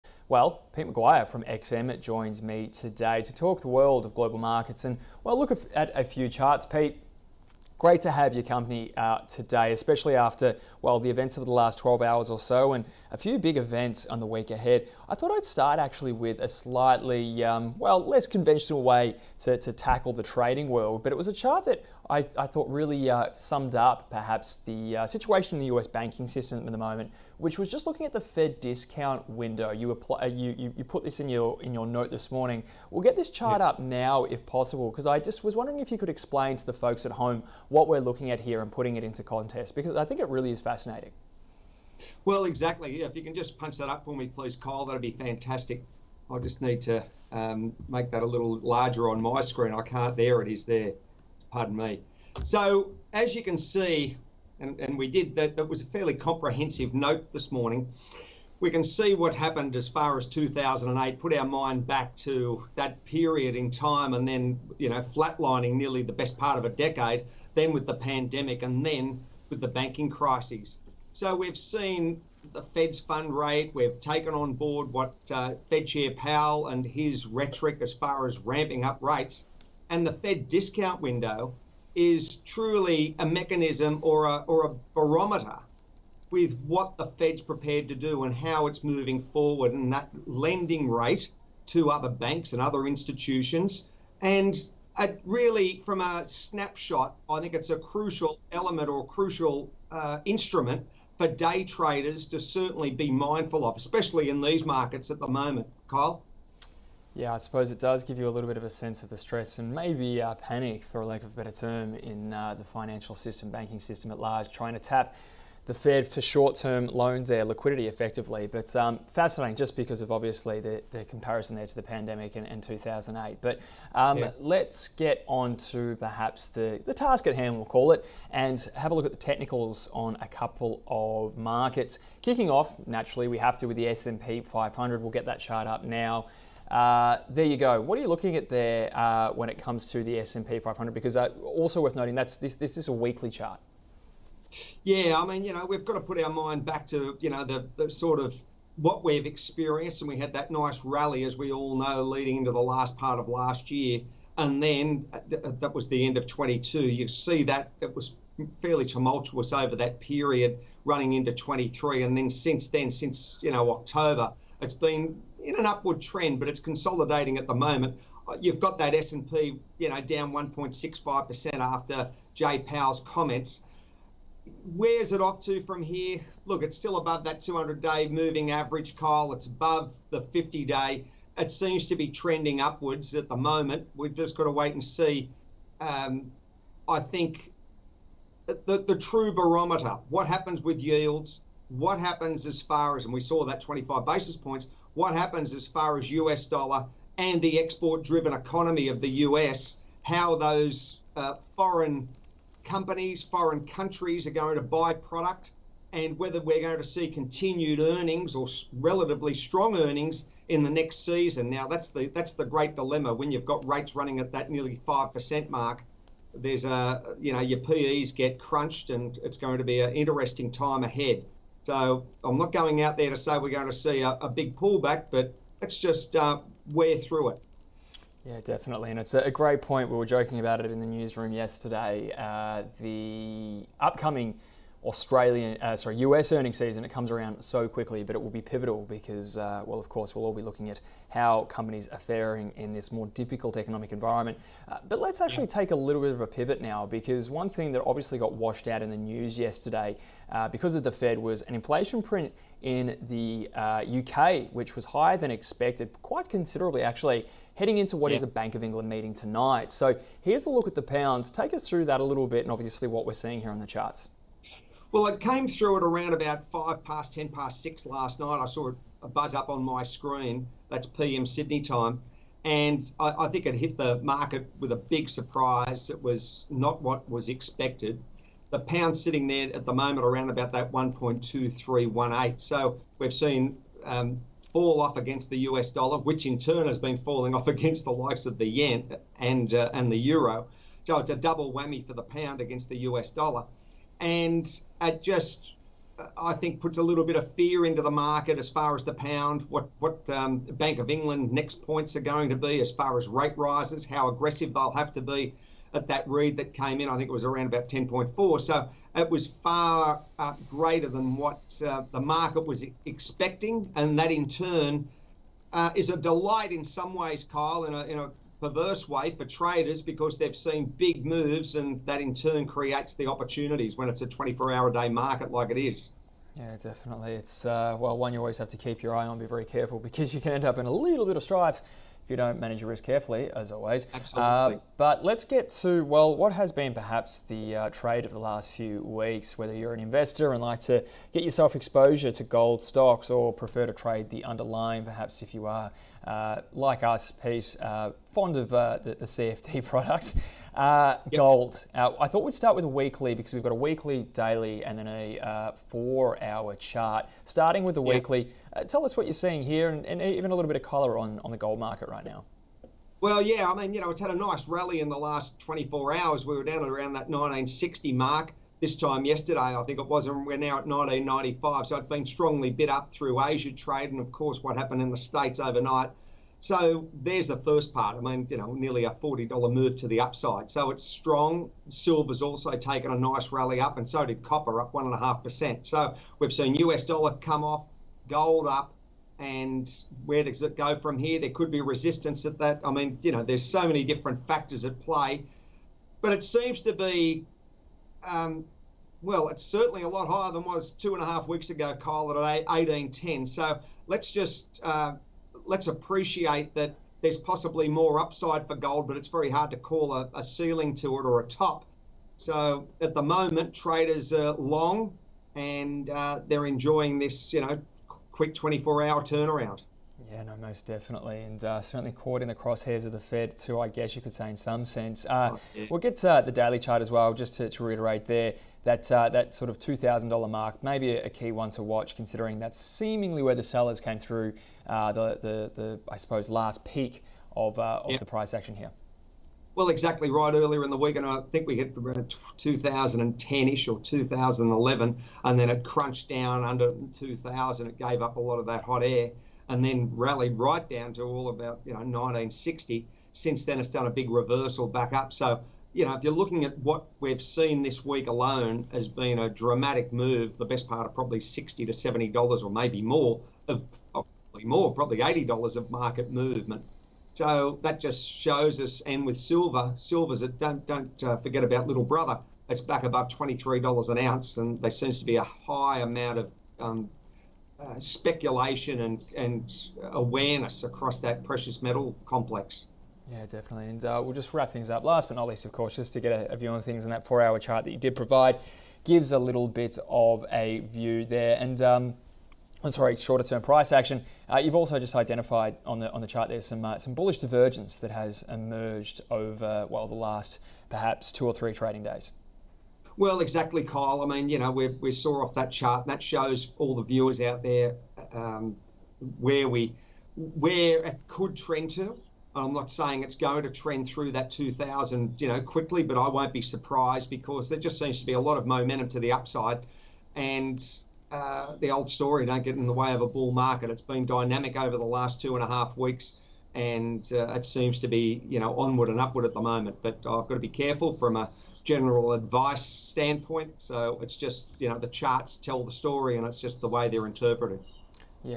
XM.COM - Peter McGuire - ausbiz - 23/03/2023
Peter McGuire XM.com speaks with ausbiz anchor Kyle Rodda.